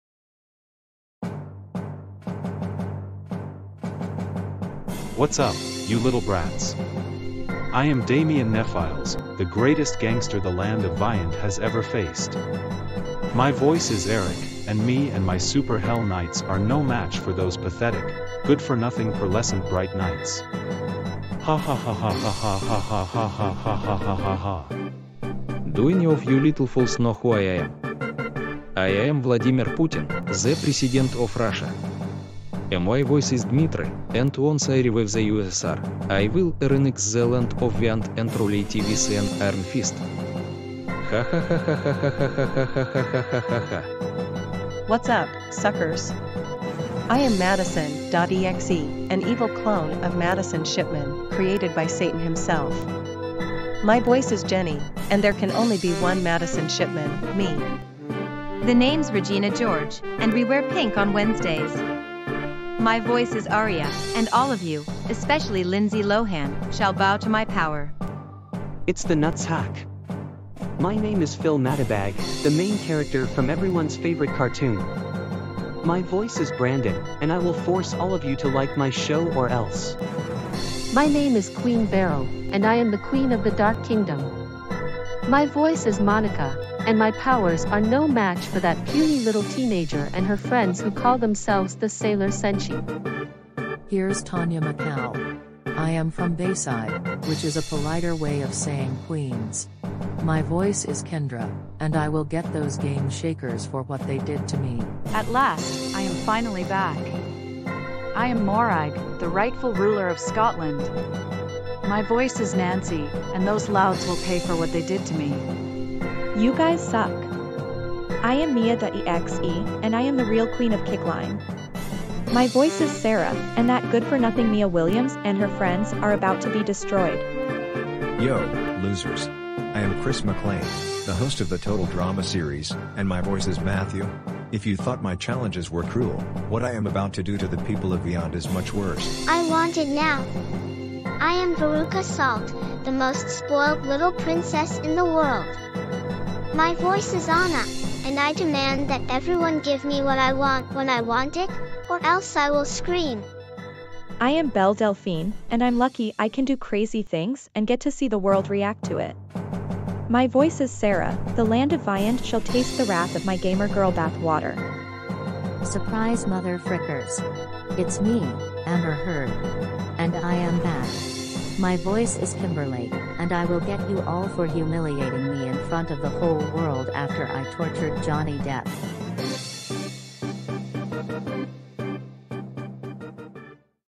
Beware the Super Hell Knights!!!
Alright, guys. So here is my villain group who will fight against my heroic team known as the Pearlescent Bright Knights. They're called the Super Hell Knights, and yes, they are just as evil as other villain groups such as the Alanastiums, the Reapers, the Legion of Mullac, the Jiminy Pack, the Legion of Superbike, etc.

Damien Nephyles, Madisyn.exe, and Mia.exe belong to yours truly.

Vladimir Putin and Amber Heard are real people.

Regina George belongs to Broadway Video.

Phil Matibag belongs to ABS-CBN International

Queen Beryl belongs to Toei Animation.

Tonya McCal belongs to Dan Schneider.

Morag belongs to Nickelodeon / Netflix.

Chris McLean belongs to Fresh TV, Inc.

Veruca Salt belongs to Roald Dahl.

Belle Delphine is a real YouTuber.